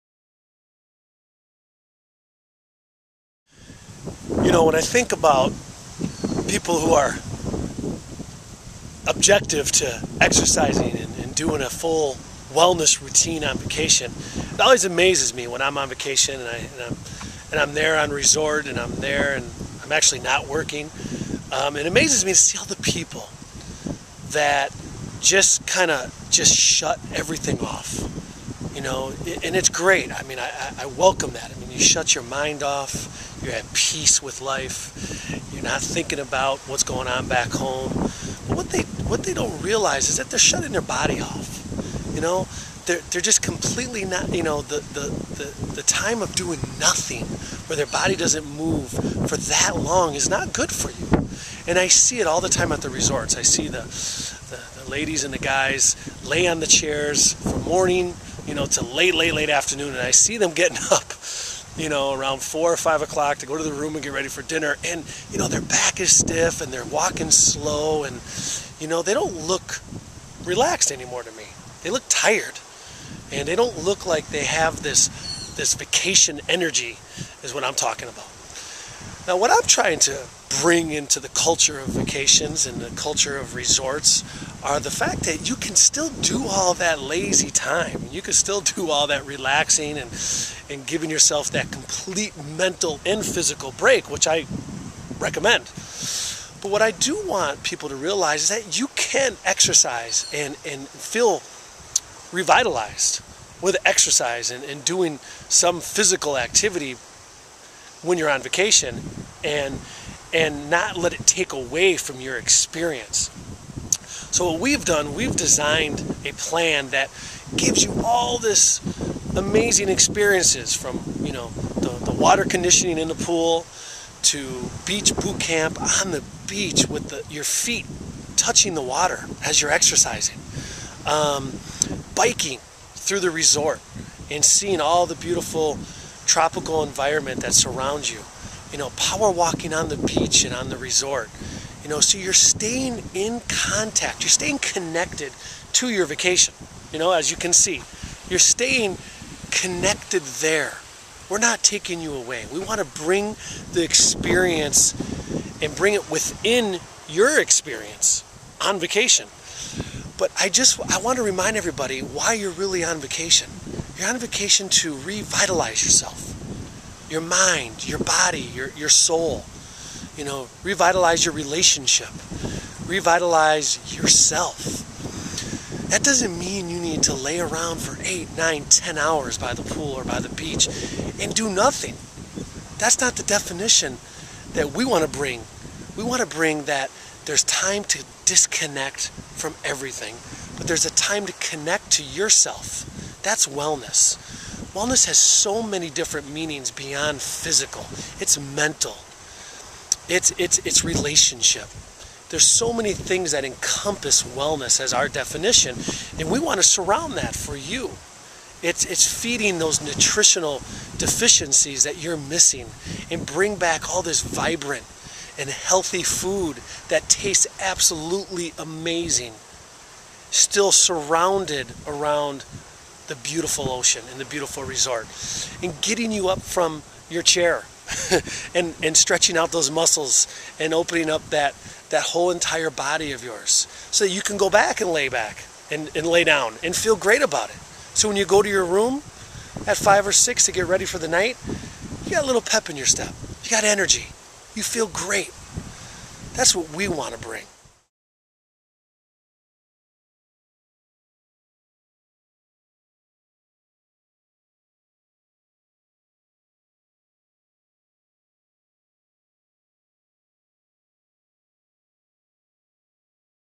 Destination Wellness - Insight from David Prichard
Inspirational words from the leader on why a healthy lifestyle should be considered at all times (Even on vacations!). When you think about people who are looking to exercise in a full wellness routine on vacation, why skip it? "Put a little pep in your step" listen up, and get ready for the lifestyle that you can celebrate by a healthier body and the pictures to prove it.